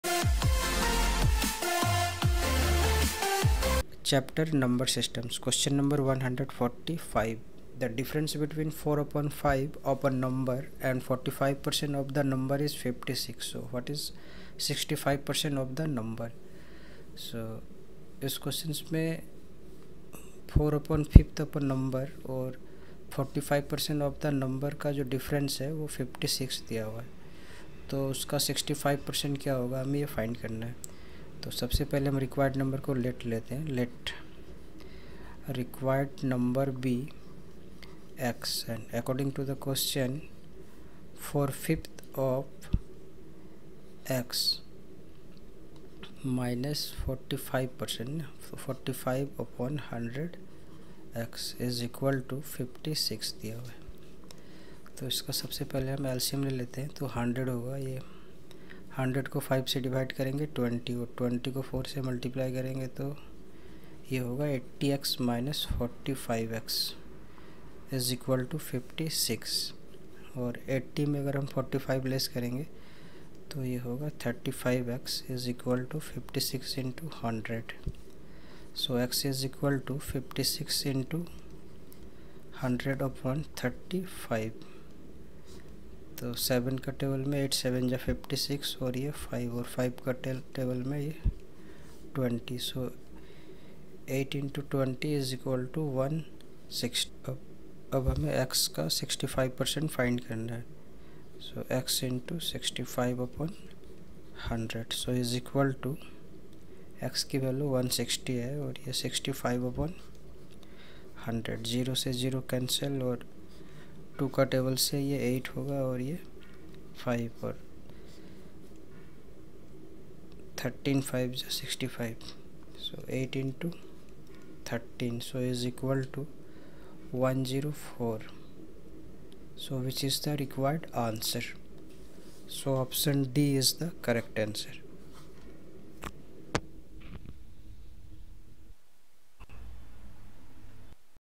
Q145 | The difference between 4/5 of a number and 45% of the number is 56. What is 65% of the number
Click on below Links for more Questions:-

Quantitative Aptitude:- NUMBER SYSTEMS
Qu(145):- The difference between 4/5 of a number and 45% of the number is 56. What is 65% of the number?